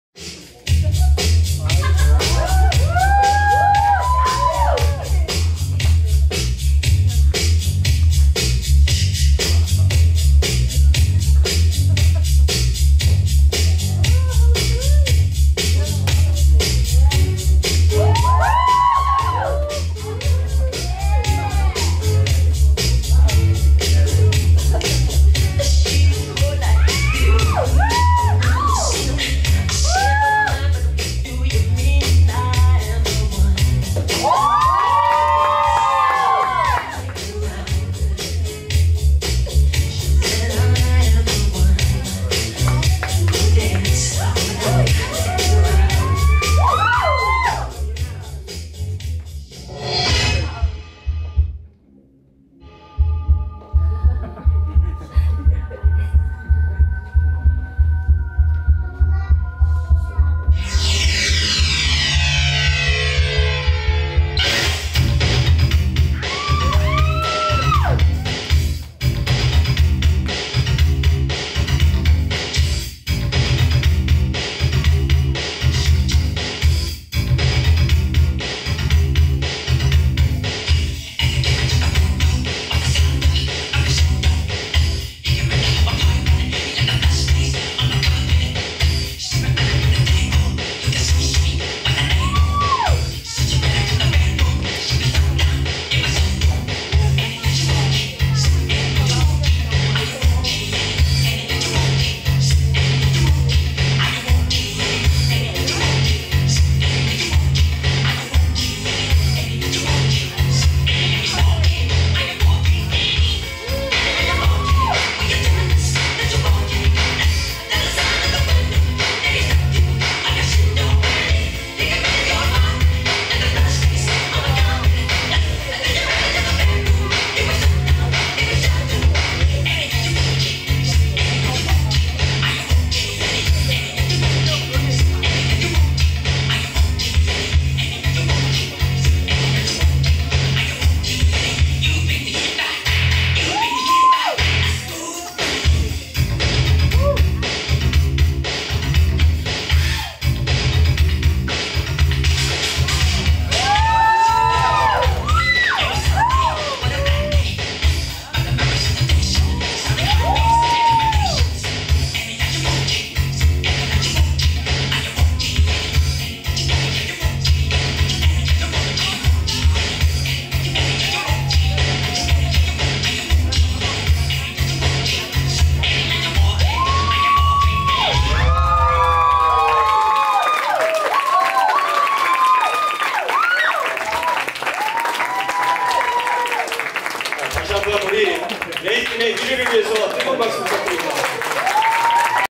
Michael Jackson dance cover by UNOS Dance Studio - 2015 Showcase
UNOS Dance Studio performs to Michael Jackson at the 2015 Christmas Showcase.